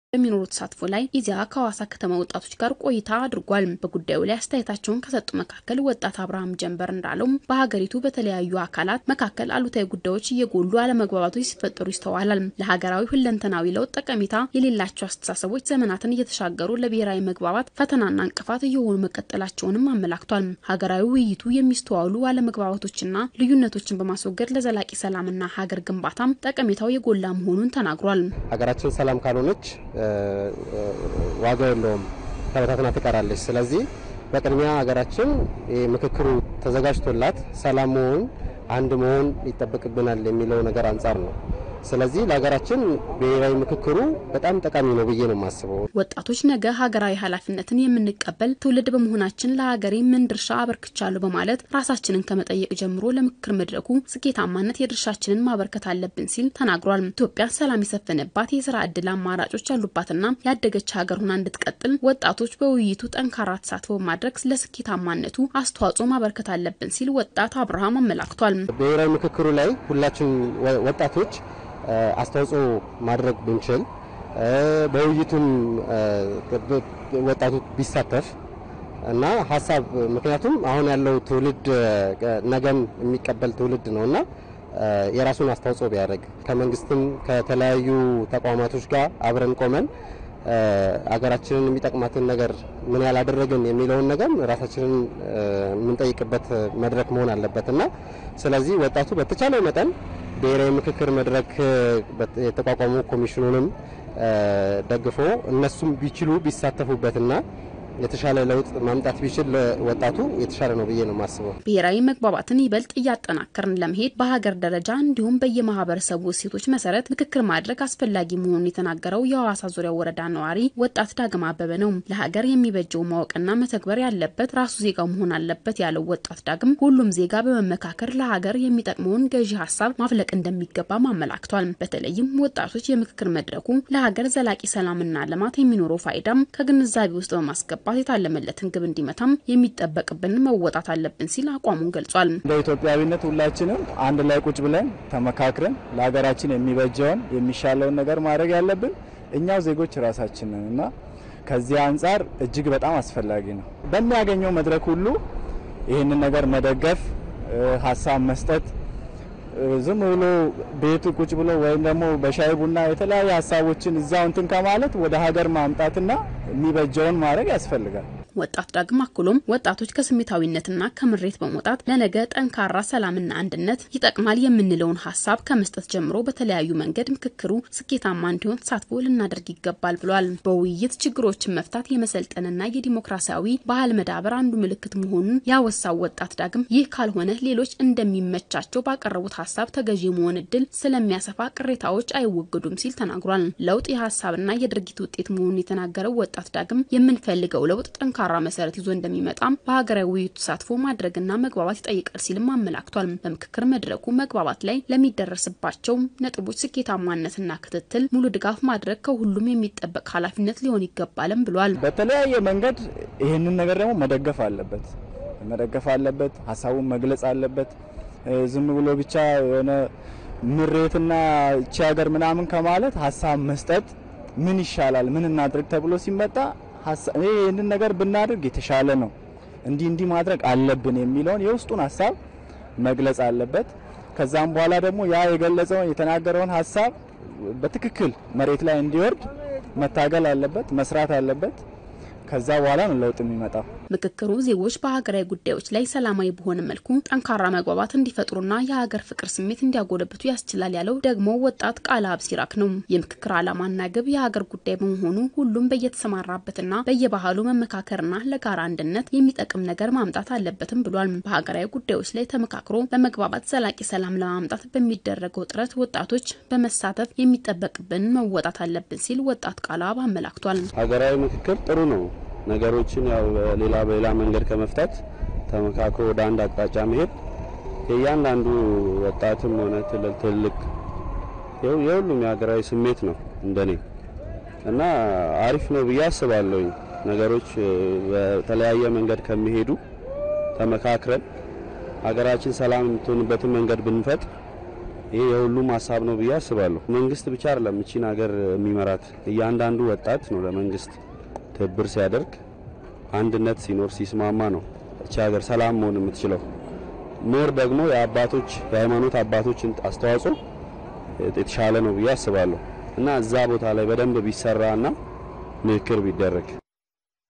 ሊካሄድ የታሰበው ሀገራዊ የምክክር መድረክ የታለመለትን ግብ እንዲመታ ወጣቶች የድርሻቸውን ሊያበረክቱ እንደሚገባ ተገለፀ
ዘገባው የኢዜአ ነው